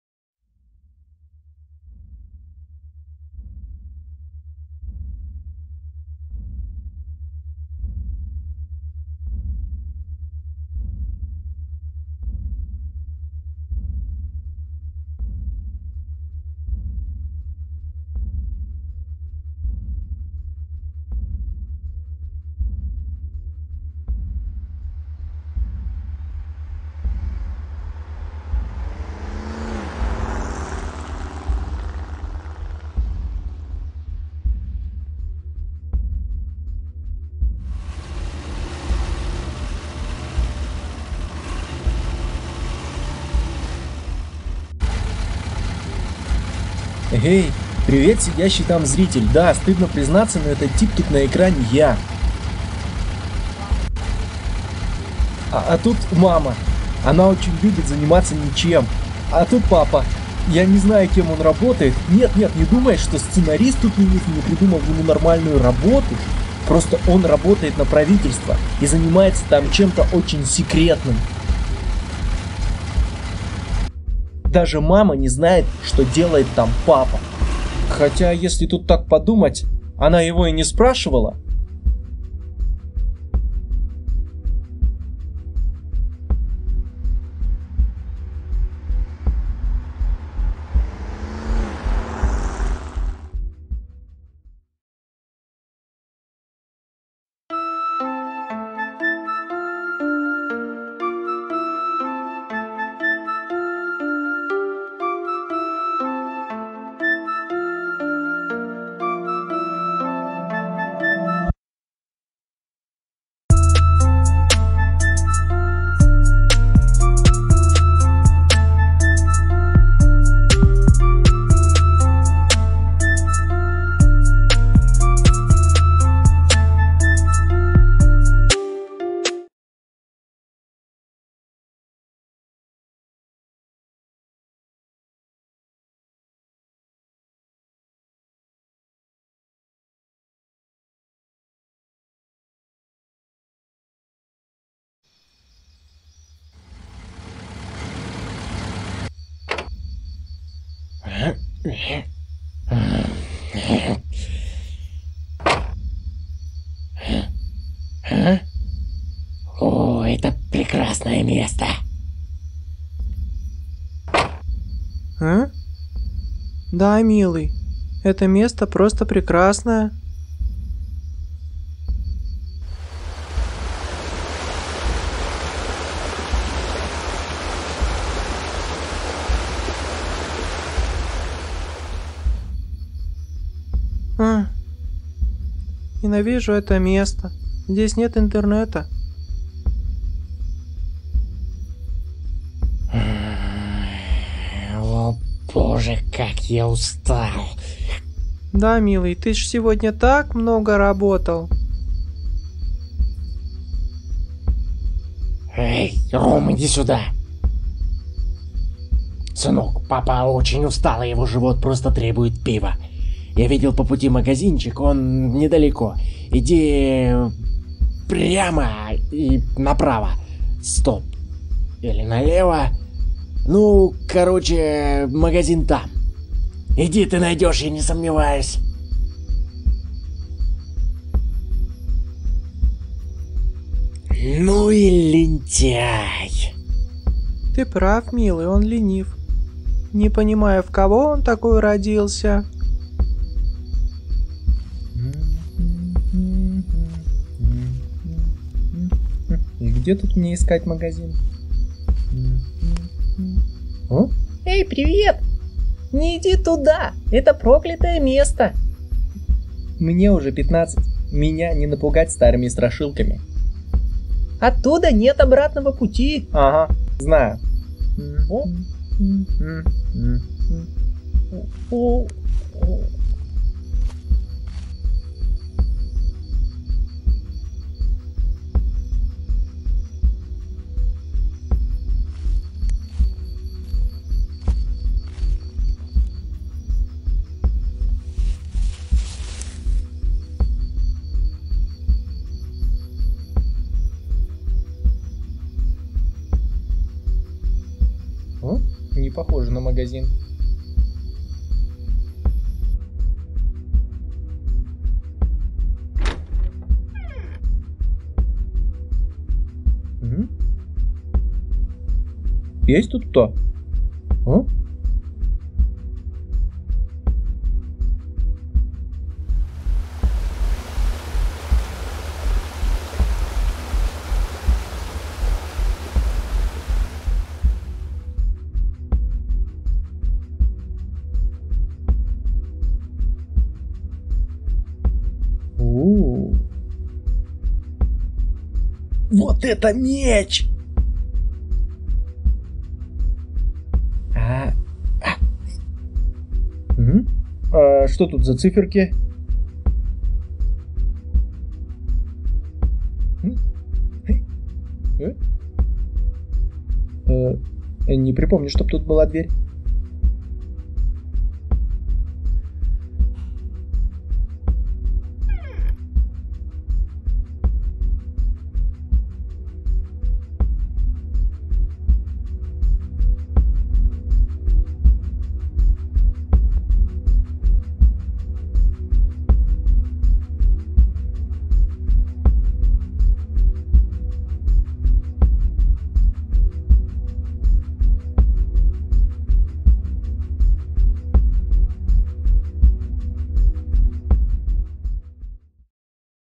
"ГоР" эпизод 1 Возвращение Домой / "GoR" || Episode 1 Homecoming
Изюменка анимации в том, что все созданно одним человеком.

 Сериал расскажет о мальчике Ром, который случайно нашел  дверь и попал в мир монстров и богов. И теперь ему надо найти путь домой.

Контакты

 Михаил Тычинин в Телеграме- @Foturneg